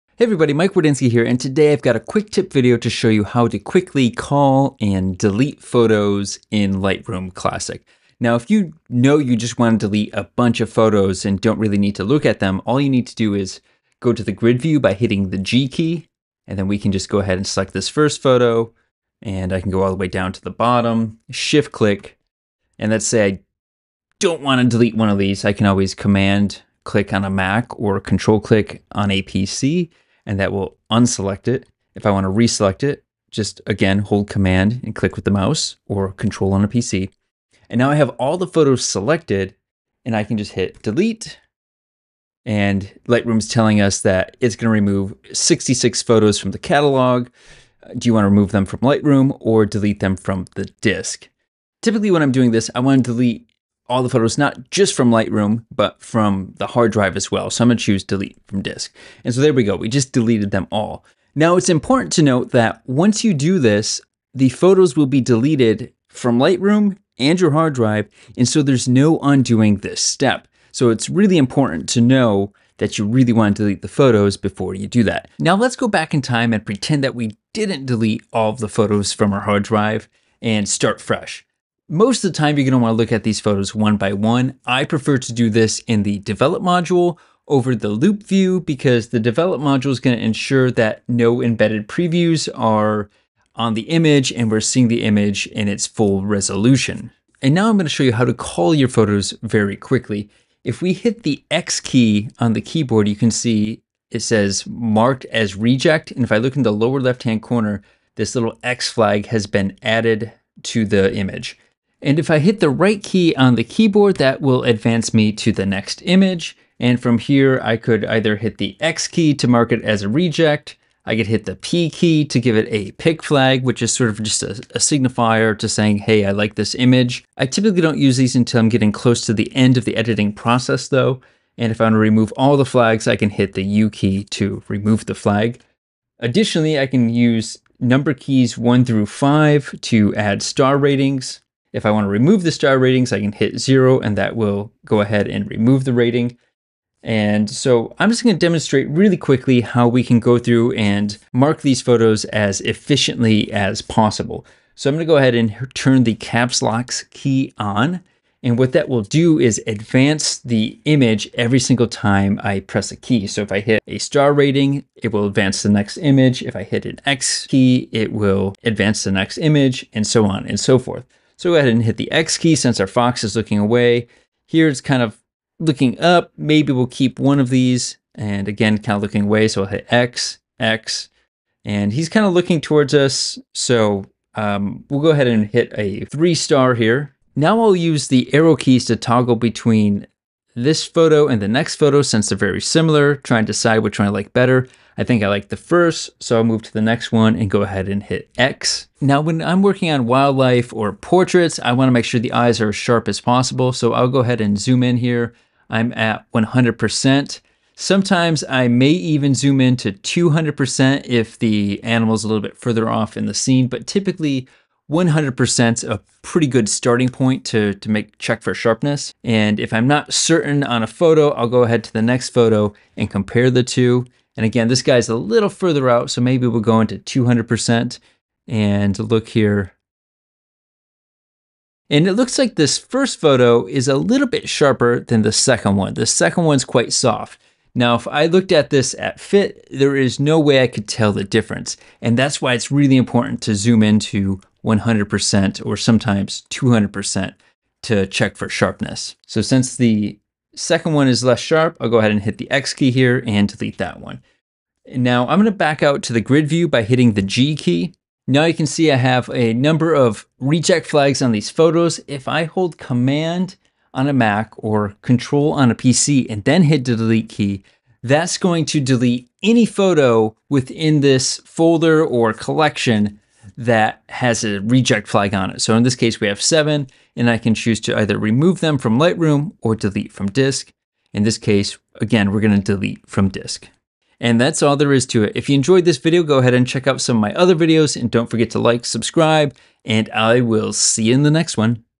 How to cull photo in Lightroom
Learn how to quickly sort and delete your photos in Lightroom Classic.

In this tutorial, you'll learn how to delete groups of photos as well as a workflow for culling and deleting photos quickly.